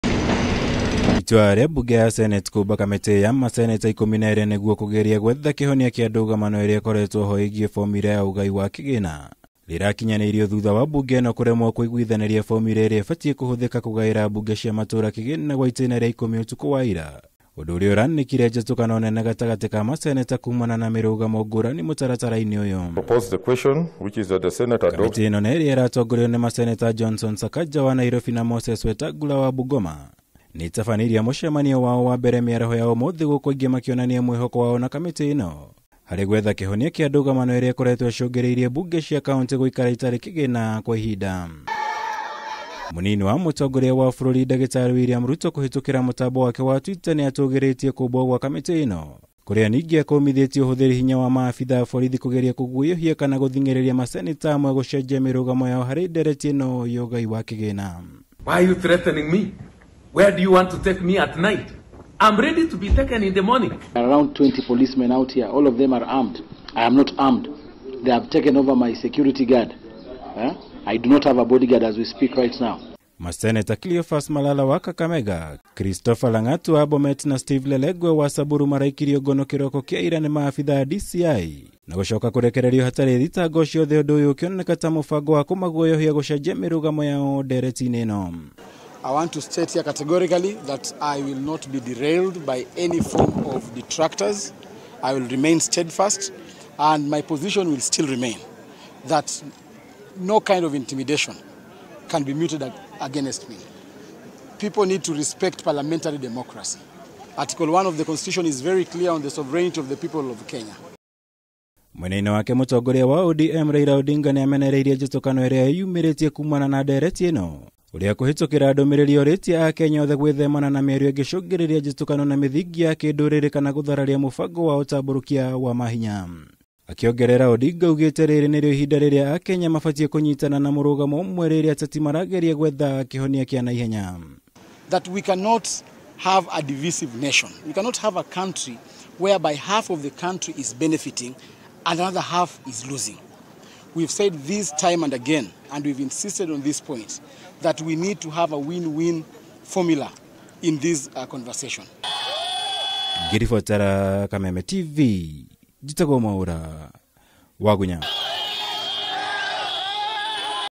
Ruto gukumithia guikio ngono kwa maseneta atatu
Munini wa mutongoria wa bururi Dr William Ruto niakumithitie uhuthiri wa maabitha a borithi kugeria kuguoyohia kana guthing’iriria atongoria kwoya murugamo umwe hari ndereti ya ugaai wa kigina, akiuga uyu niundu muhituikite ni mahinda na tiguo mathuriirwo ni muingi.
Riri mutongoria wa ODM Raila Odinga akumithitie njatukano iria ikoretwo ciiyumiritie kuumana na ndereti ino ya ugaai wa kigina, na uguo akoria atongoria kuhorohiua mirugamo yao miruru na niguo gwetha kihonia kia naihenya.
Na ta uria Mwaura wa Gunya aratutariria, kamiti ya maseneta 12 iria irombirwo kumurika undu uyu niitabanirie mucemanio wayo wa mbere umuthi.